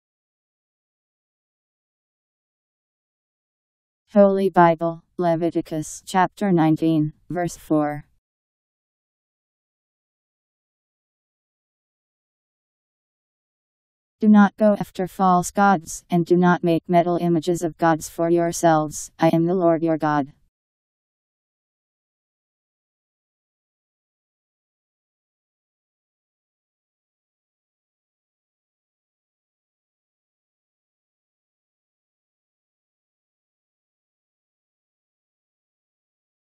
Holy Bible Leviticus Chapter 19 Verse 4
Video shows Holy Bible Leviticus Chapter 19 Verse 4: Do not go after false gods, and do not make metal images of gods for yourselves: I am the Lord your God. Powered by MaryTTS. Text taken from Bible in Basic Edition